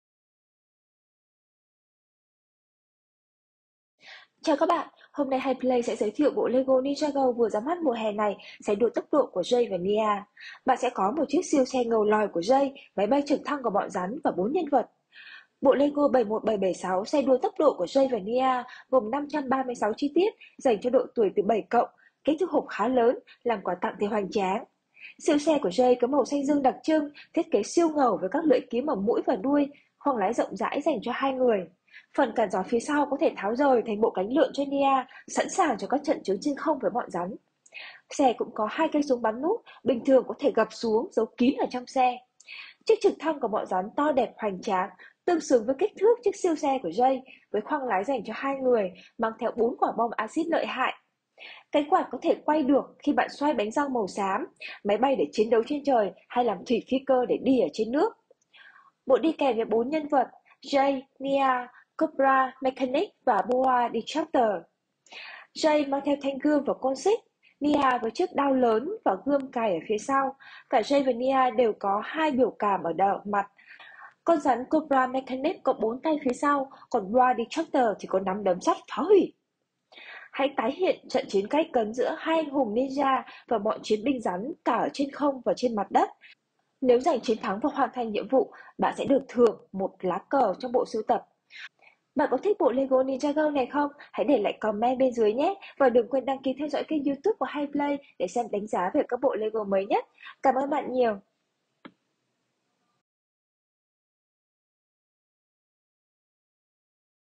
Đánh giá LEGO NINJAGO 71776 Xe đua tốc độ của Jay và Nya
Mua đồ chơi LEGO NINJAGO 71776 Jay and Nya's Race Car EVO
🌳 Giá: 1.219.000 (Tiết kiệm: 30%). LEGO Chính hãng. Giao hàng 2h*. Mua tại - Hệ thống cửa hàng bán đồ chơi LEGO tại TPHCM và Hà Nội.